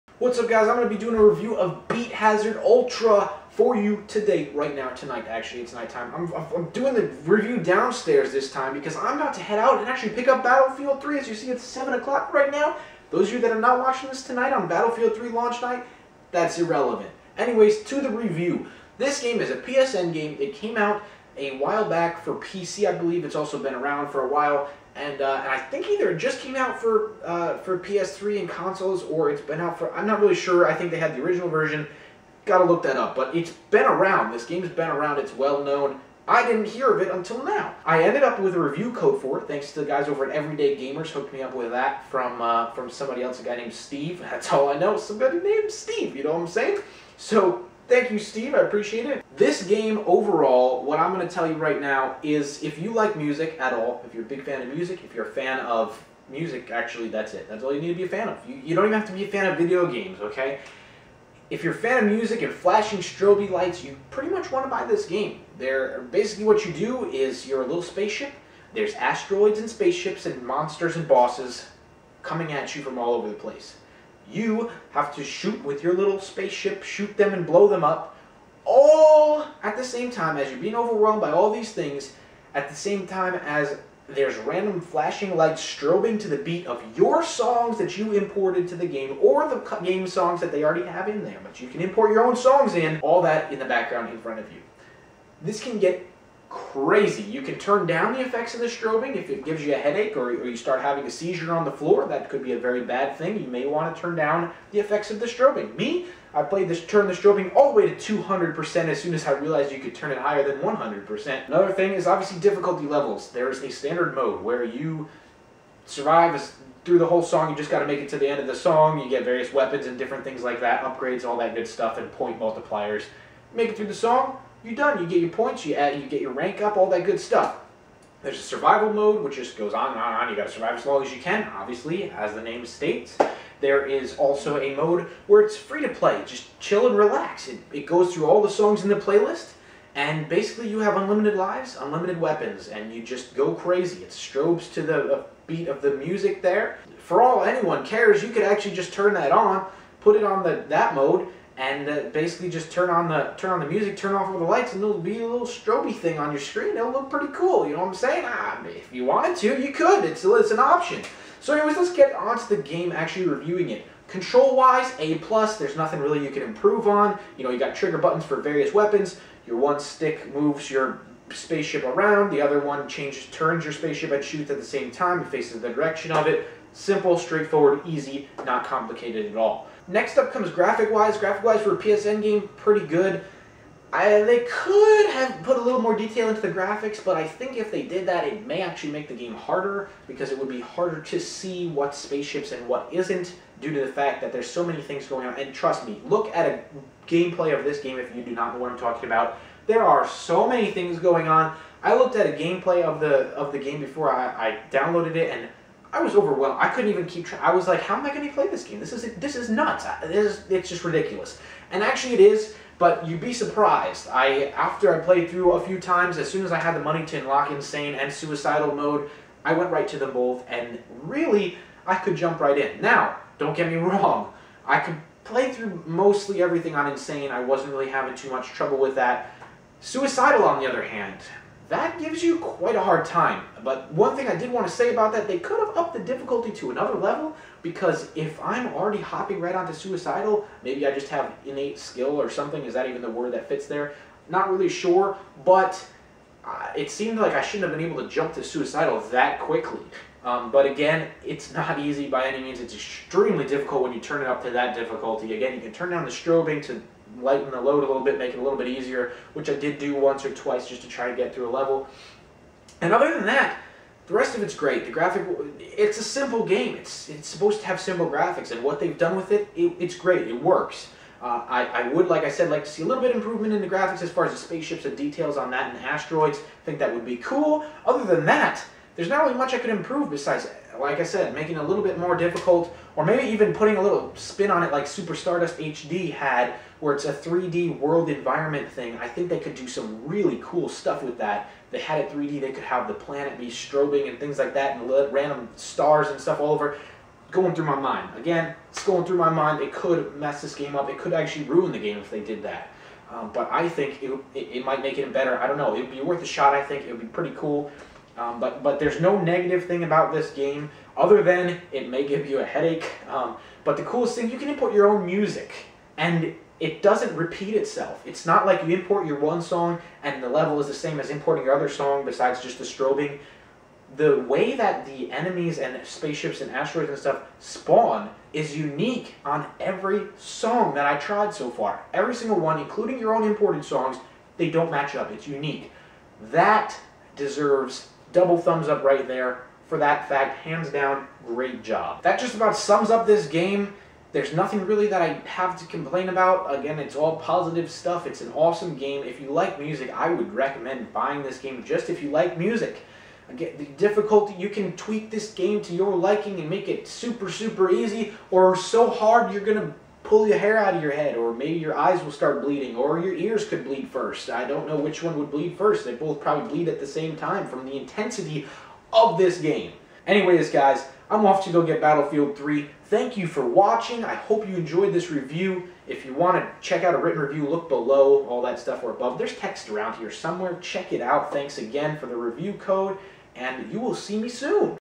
Beat Hazard Ultra Game Review - PSN - XBL - PC
Beat Hazard Ultra was released for consoles on October 19th, 2011.

Personally, I had never heard of this game before, but I ended up with it and I can wholeheartedly say that I am extremely glad I did!

The best way that I can describe Beat Hazard Ultra is to take superstardust HD, put all your favorite dance/electro/dubstep music behind it and put thousands of music strobing lights on the screen at the same time.

When I first looked at a gameplay video of this game I couldn't even keep track of what was going on, but soon after I downloaded it, I was already playing on Suicidal and it was pure fun!

Overall, graphicswise I would say that it was pretty well done. I would have liked to see a little more detail on the various ships and bosses, but by no means does this deter me from playing Beat Hazard Ultra.

One thing that did draw me into this game was the fact that you could import your own music and Beat Hazard Ultra would create a level based off of your songs. As far as I could tell, it seemed to be unique for every song that I imported, so it didn't get repetitive.

Also, as most of you know, I'm a huge multiplayer fan and I was extremely pleased to know that Beat Hazard Ultra supported multiplayer and not only on the same console, but it also has online multiplayer capabilities!

Overall, if I could improve one thing on this game, I would probably put a higher difficulty mode on it. I found that even though Suicidal Game Mode  was by no means easy, I was able to jump right into it after just a few playthroughs on some of the lower difficulty settings.

One other thing that could possibly improve or destroy this game, would be to add the 3-D world around it, such as in Superstardust HD. I think this would allow for a lot more difficulty and also an even more insane user experience regarding all the things that could be done with the strobing effects.

After all this being taken into consideration, I believe that if you love music and you're a gamer, Beat Hazard Ultra is worth looking into. Not only do you have the option of playing through to beat your highscores, but there is an infinite mode, where you can just relax and listen to your music and destroy thousands and thousands of spaceships and asteroids coming from every direction without the pressure of trying not to die.